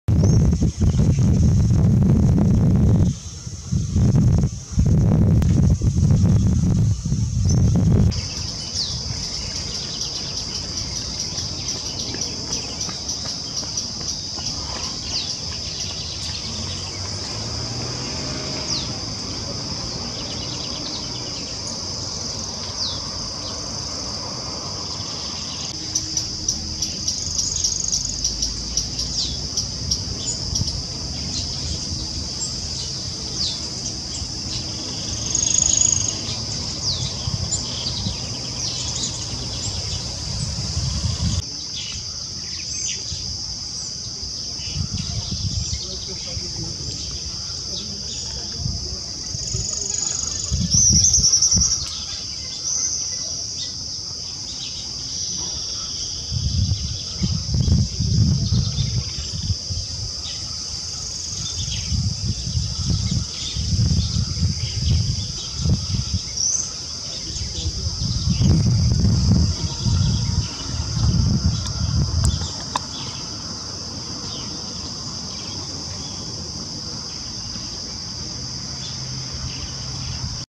PICA PAU ANÃO BARRADO - Dunga e suas lentes (abril de 2021)
Esse é um registro que gosto muito desse pica-pau. São dóceis e fáceis de serem filmados. Filmei na Lagoa de Imboassica aqui em Macaé (RJ). Uma furadeira com penas! Penso eu e respeito que lugar de pássaros é fora de gaiolas. Corruptos sim, esses precisam de um recolhimento e reflexões.